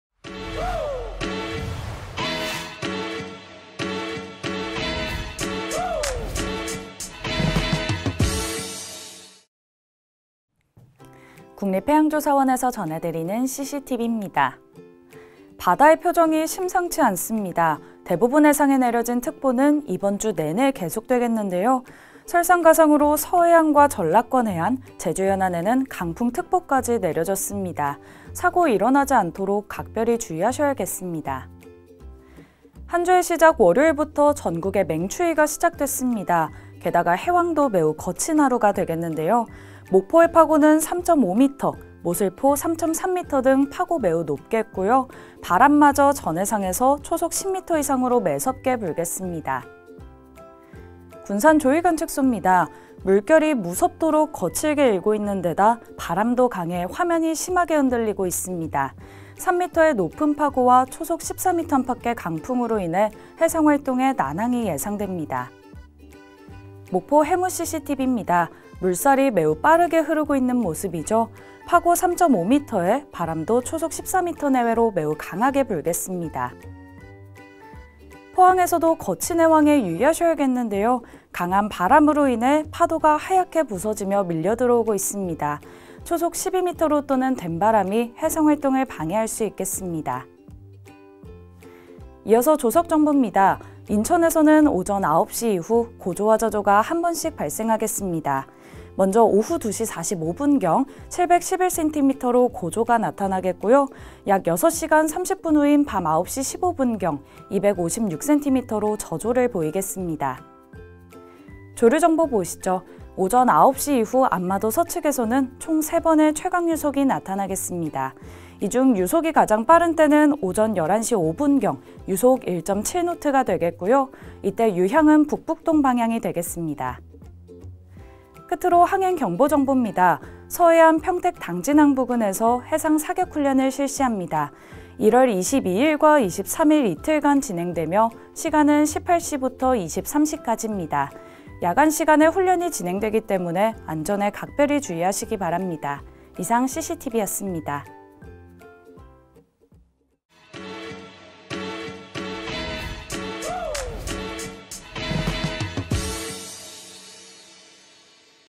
[See Sea TV] 24년 1월 22일 바다의 표정이 심상치 않습니다! 대부분 해상에 내려진 특보는 이번 주 내내 계속‥/ 해양예보 On바다
국립해양조사원에서 전해드리는 sstv입니다
바다의 표정이 심상치 않습니다
대부분 해상에 내려진 특보는 이번 주 내내 계속되겠는데요
설상가상으로 서해안과 전라권 해안, 제주연안에는 강풍특보까지 내려졌습니다
사고 일어나지 않도록 각별히 주의하셔야겠습니다

한 주의 시작 월요일부터 전국에 맹추위가 시작됐습니다
게다가 해황도 매우 거친 하루가 되겠는데요
목포의 파고는 3.5m, 모슬포 3.3m 등 파고 매우 높겠고요
바람마저 전 해상에서 초속 10m 이상으로 매섭게 불겠습니다

군산 조위관측소입니다
물결이 무섭도록 거칠게 일고 있는데다,
바람도 강해 화면이 심하게 흔들리고 있습니다
3m의 높은 파고와 초속 14m 안팎의 강풍으로 인해
해상활동에 난항이 예상됩니다

목포 해무cctv입니다
물살도이 매우 빠르게 흐르고 있는 모습이죠
파고 3.5m에 바람도 초속 14m 내외로 매우 강하게 불겠습니다

포항에서도 거친 해황에 유의하셔야겠는데요
강한 바람으로 인해 파도가 하얗게 부서지며 밀려들어오고 있습니다
초속 12m를 웃도는 된바람이 해상활동을 방해할 수 있겠습니다

이어서 조석정보입니다
인천에서는 오전 9시 이후 고조와 저조가 한 번씩 발생하겠습니다
먼저 오후 2시 45분경, 711cm로 고조가 나타나겠고요
약 6시간 30분 후인 밤 9시 15분경, 256cm로 저조를 보이겠습니다

조류정보 보시죠
오전 9시 이후 안마도 서측에서는 총 3번의 최강유속이 나타나겠습니다
이 중 유속이 가장 빠른 때는 오전 11시 5분경,
유속 1.7노트가 되겠고요
이때 유향은 북북동 방향이 되겠습니다

끝으로 항행경보 정보입니다
서해안 평택당진항 부근에서 해상사격훈련을 실시합니다
1월 22일과 23일 이틀간 진행되며, 시간은 18시부터 23시까지입니다
야간 시간에 훈련이 진행되기 때문에 안전에 각별히 주의하시기 바랍니다
이상 sstv였습니다

- 고보정 해양캐스터